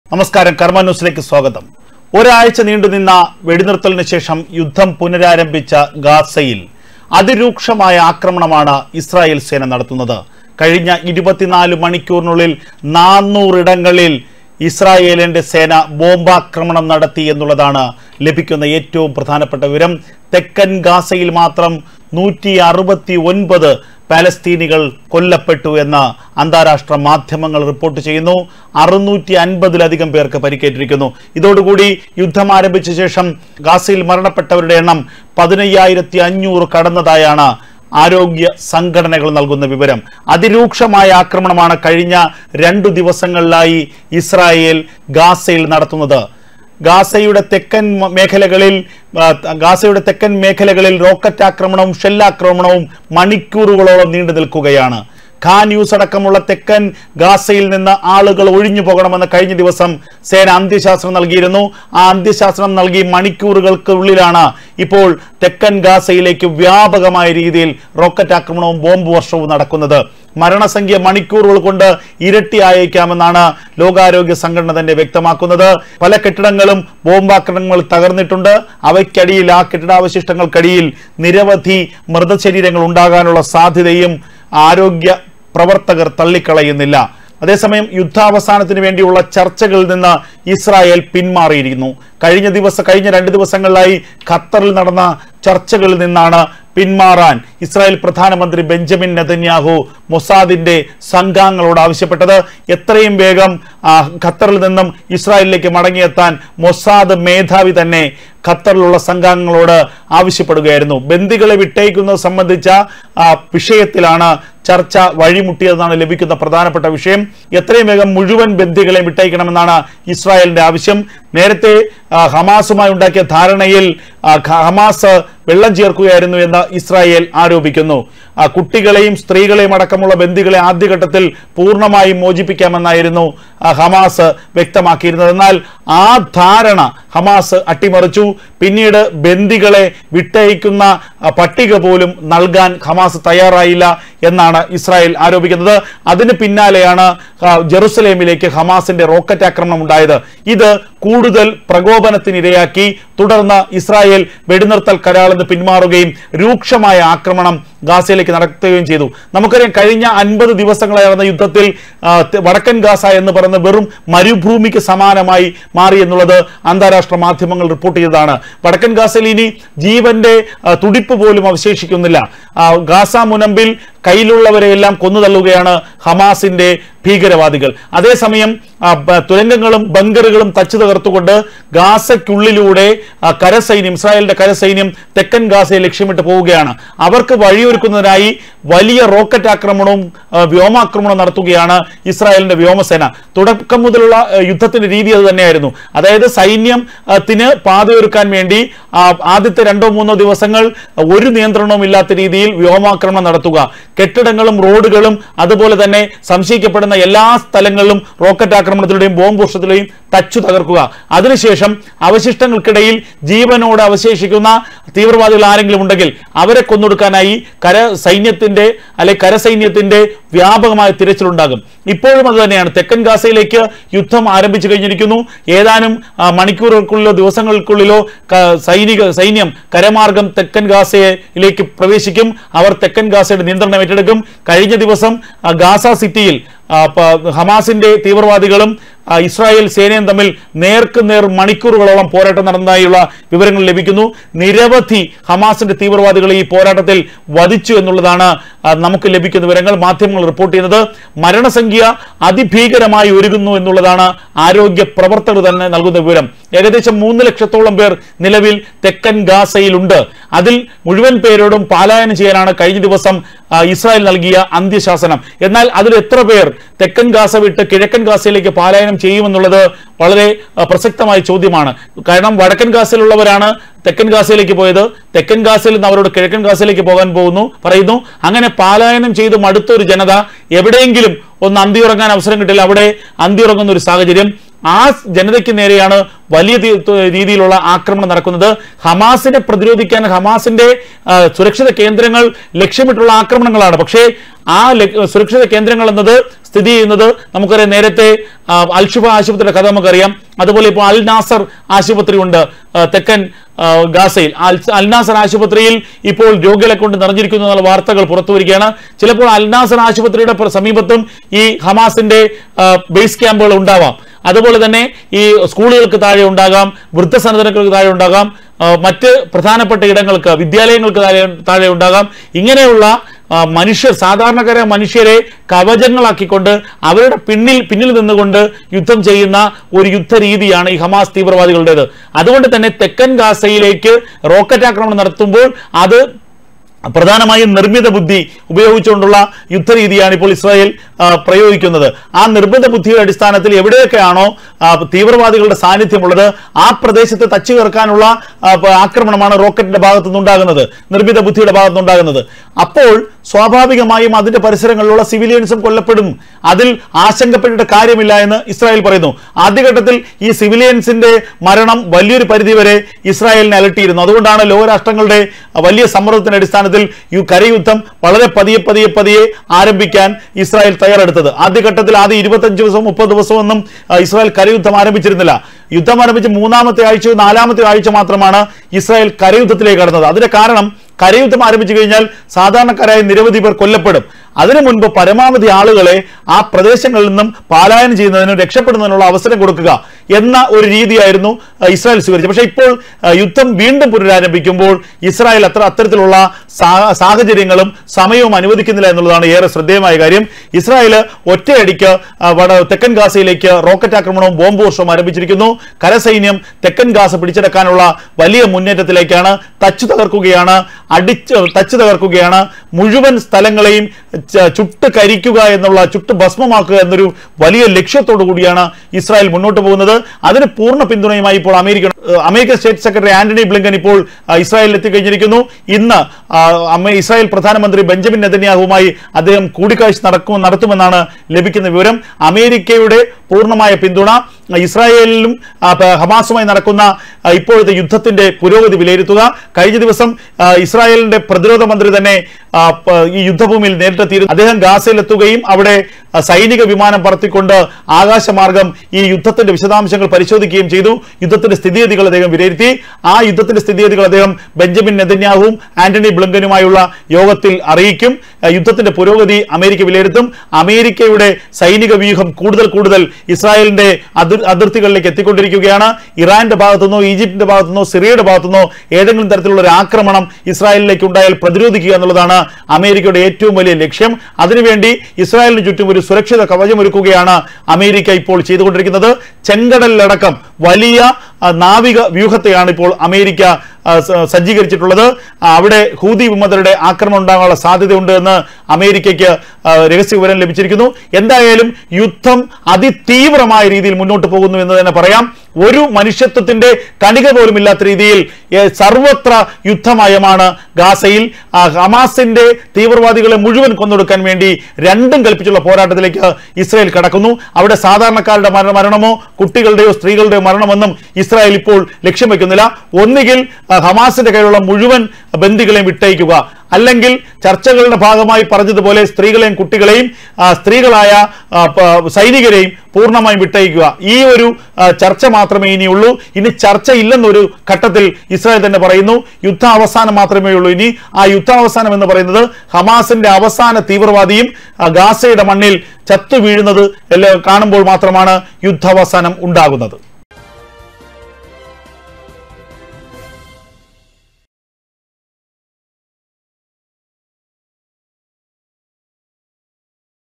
ജൂതപ്പടയുടെ കൊലവിളി,400 ഇടങ്ങളിൽ ബോംബ് വർഷം, ഗാസ ചിന്നിച്ചിതറി
ഇനി ചരിത്രത്തിലെ ഭയാനക യുദ്ധം, 24 മണിക്കൂറിനിടെ 400 ഇടങ്ങളിൽ ബോംബിട്ടു, ഇന്ന് മാത്രം കൊല്ലപ്പെട്ടത് 200 ലധികം പേർ